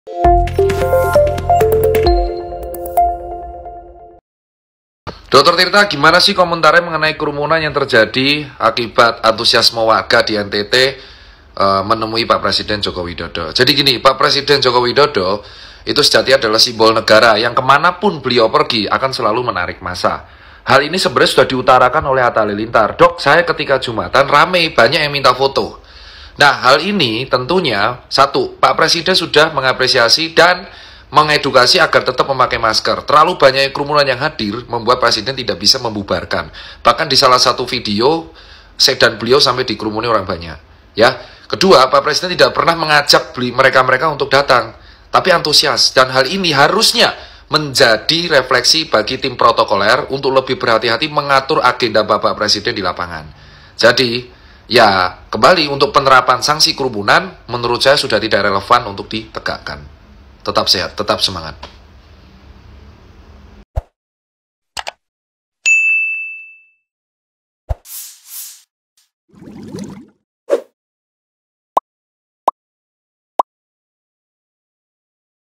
Dokter Tirta Tanggapi Kerumunan Jokowi di NTT
Relawan peduli Covid-19, Dokter Tirta memberikan tanggapan terkait kerumunan Presiden Jokowi di NTT.

Menurutnya sanksi kerumunan sudah tidak relevan ditegakan pada kasus kerumunan Presiden Jokowi.

Video ini dibagikan di akun Tiktok pribadinya @tirtacipeng, Rabu (24/2/2021).

Sebelumnya, kerumunan Presiden Jokowi jadi sorotan lantaran terjadi di tengah pandemi corona.

Video terkini dan terbaru..